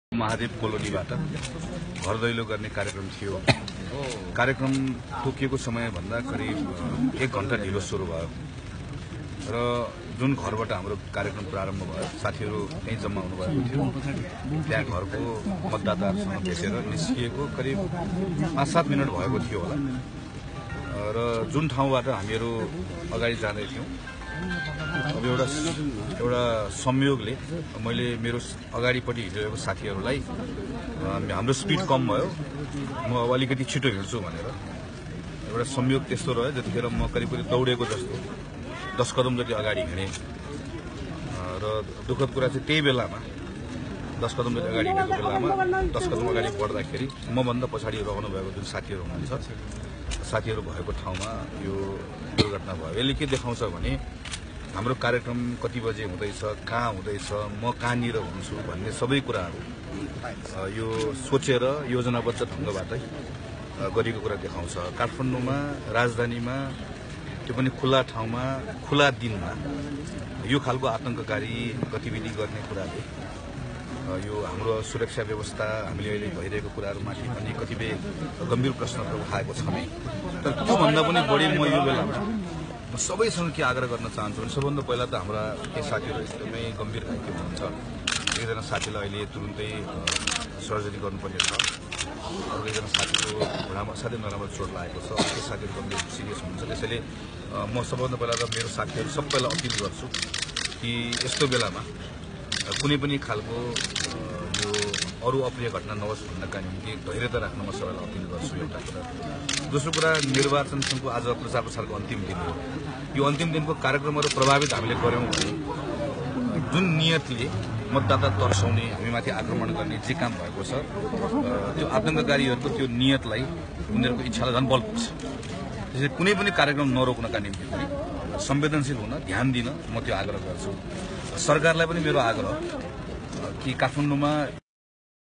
Leader Gagan thapa after discharge from hospital. He was injured 🤕 bg Bom blast
Leader Gagan thapa